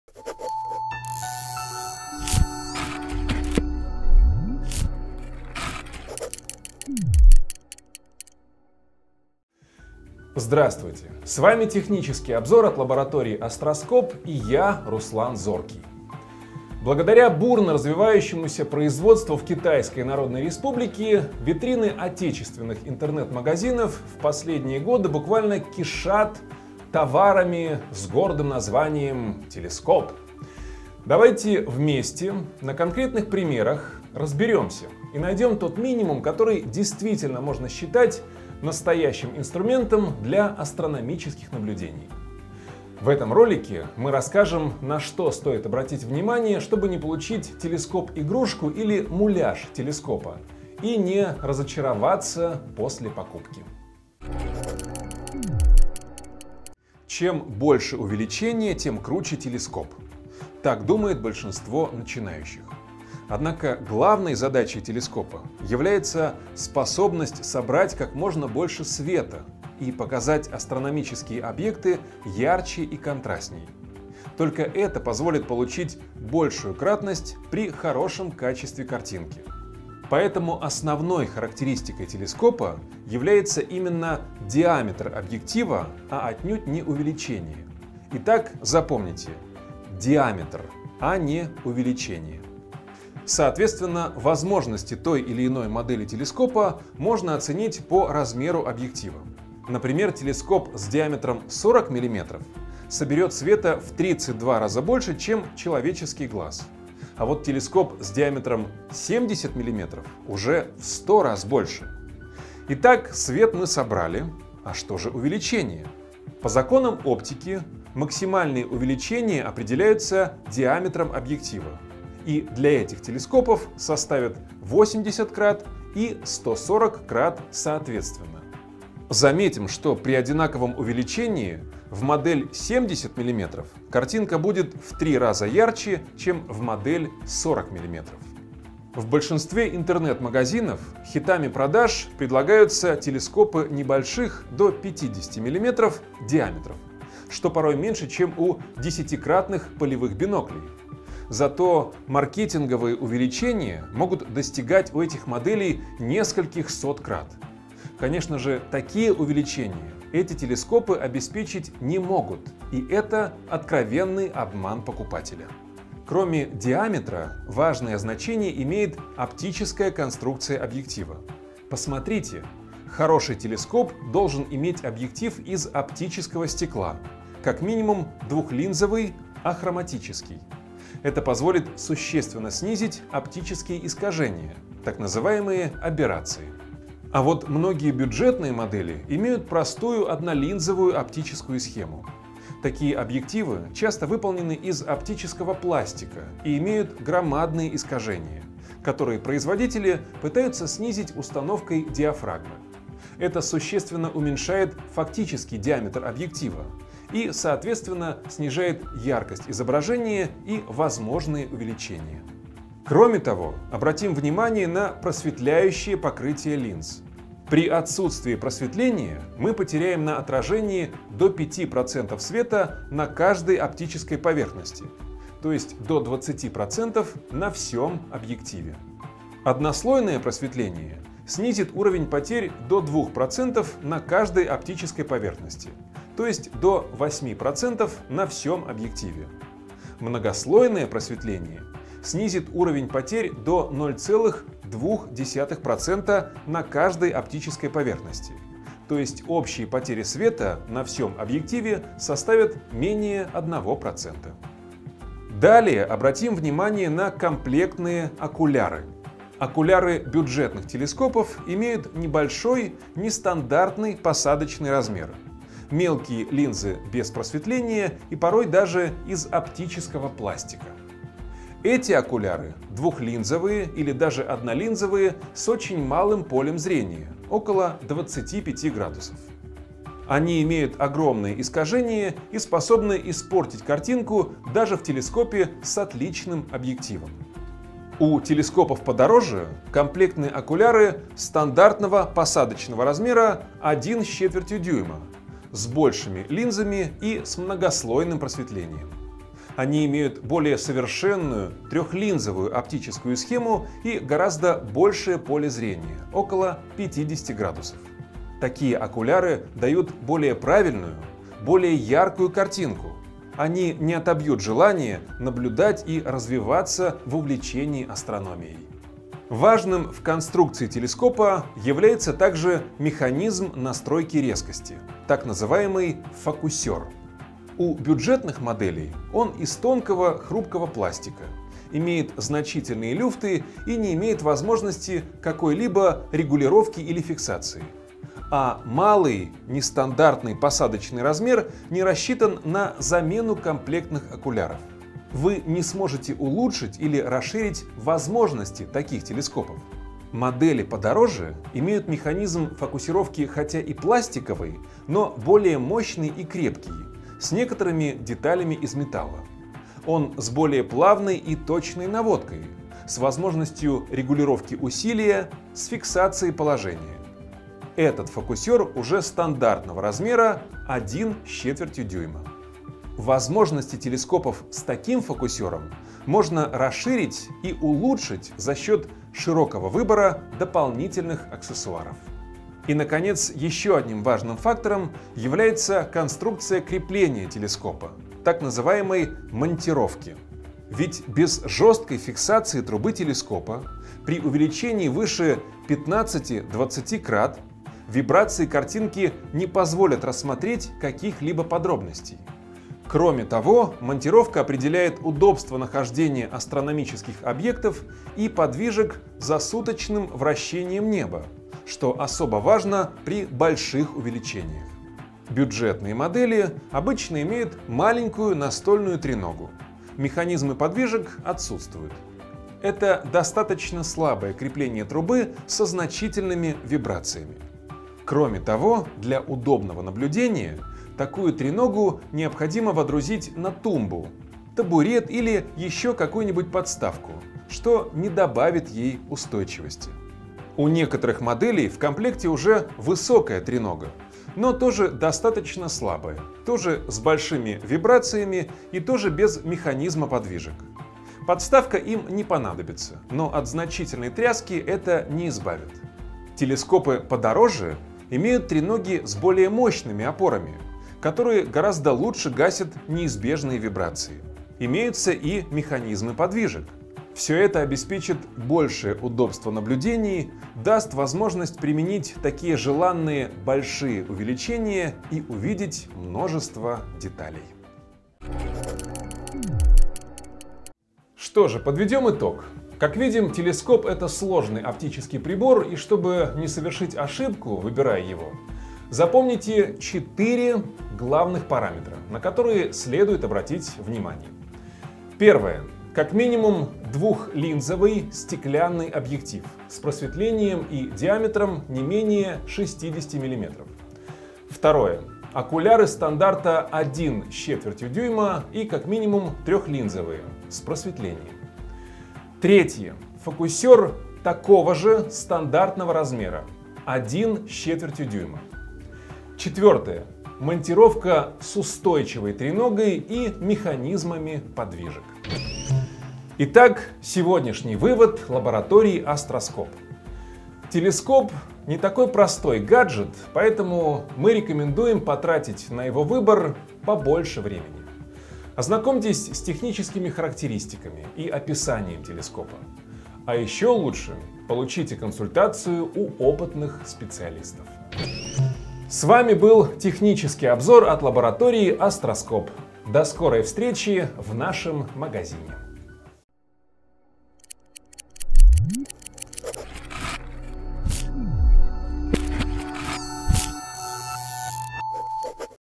Как выбрать первый телескоп
В этом ролике мы расскажем на что стоит обратить внимание, чтобы не получить телескоп-игрушку или муляж телескопа, и не разочароваться после покупки.